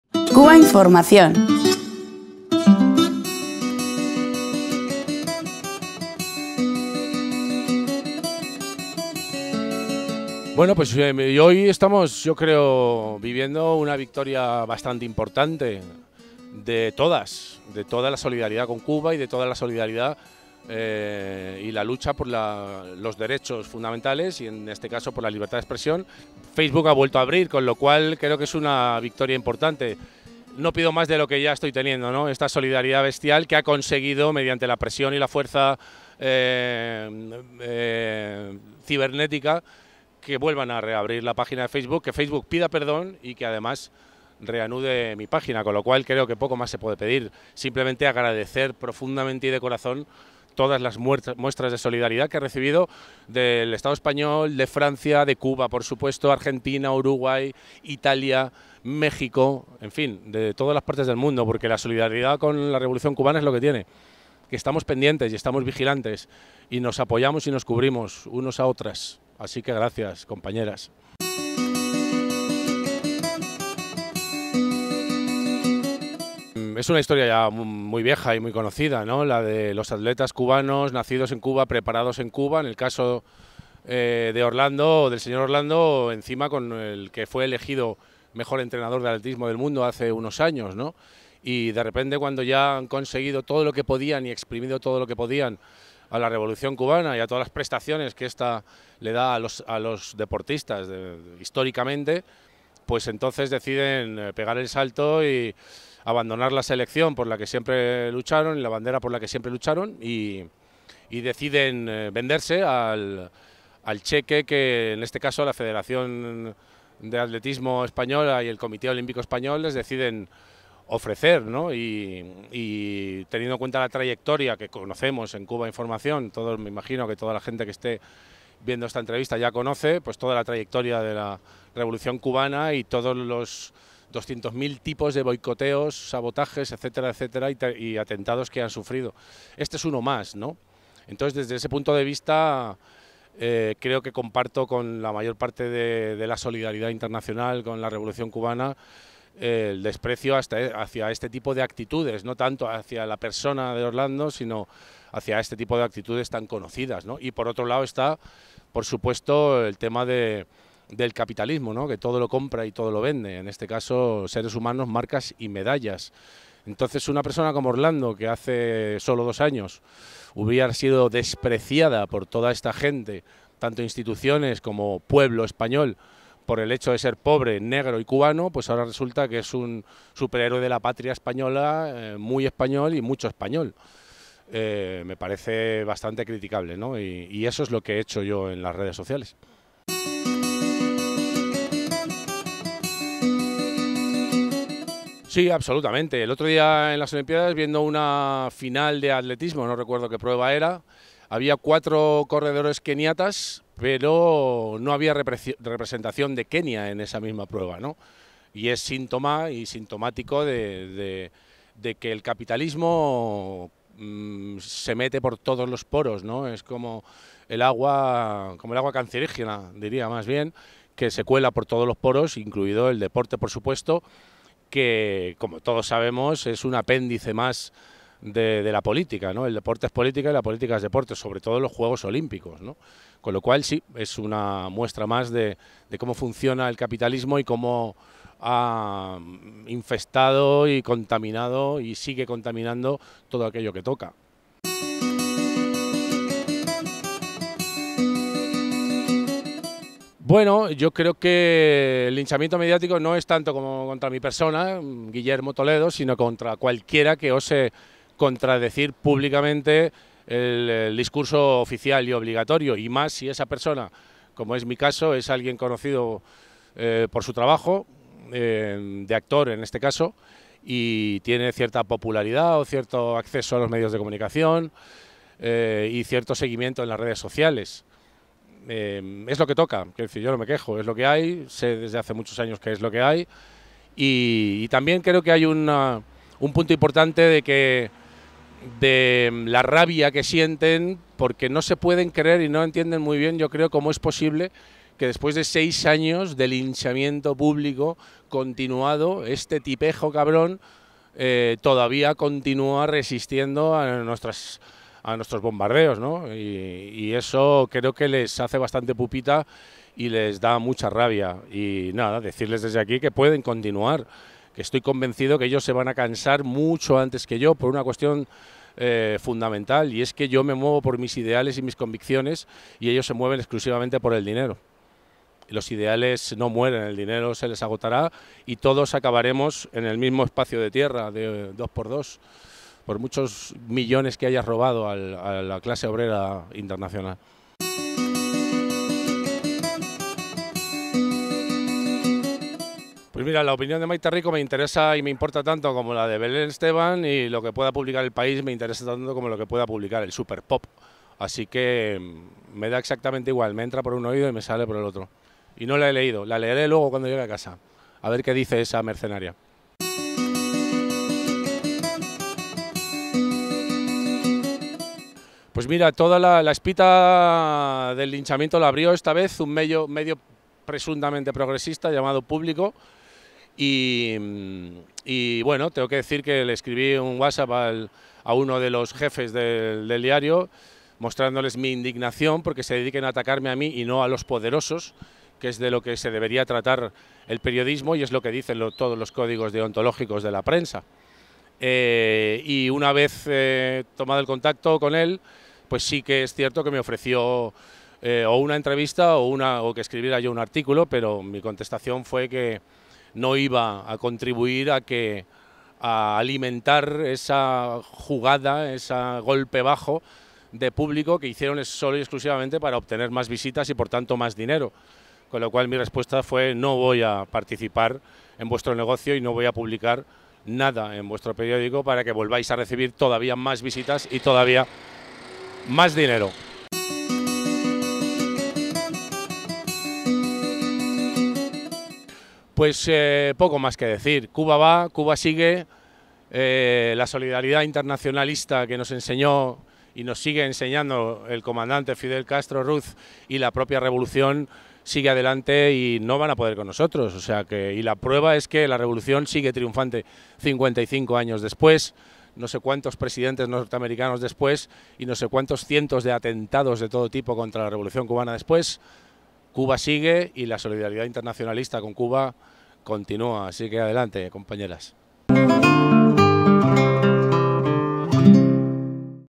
Willie Toledo: `La solidaridad con Cuba ha hecho que Facebook reabra mi página censurada´
En declaraciones en exclusiva para Cubainformación TV, desde el Aeropuerto de Barajas (Madrid), el actor y activista Guillermo Toledo responde al nuevo linchamiento mediático que sufre, tras sus críticas al tratamiento informativo sobre la medalla de plata obtenida para España por el atleta formado en Cuba Orlando Ortega, en las pasadas Olimpiadas.

Preguntamos al actor si el caso Ortega es ejemplo de un modelo deportivo injusto para los países más pobres.

También sobre las constantes campañas de ataque a su persona desde los grandes medios. "Les da una rabia inmensa y no se pueden creer que, después de seis años de linchamiento continuado este tipejo cabrón continúe resistiendo", afirma con orgullo.

"Pero se van a cansar. Porque yo actúo solo por ideales y ellos... por dinero".

Sobre artículistas como Maite Rico que le han insultado, sin derecho a réplica, desde las páginas de poderosos medios como "El País", sencillamente dice que es una "mercenaria".

Finalmente agradece la inmensa solidaridad de tantas personas que le han apoyado desde México, Cuba, España, Argentina y otros lugares del mundo.

Cámara: Iñigo Fernández.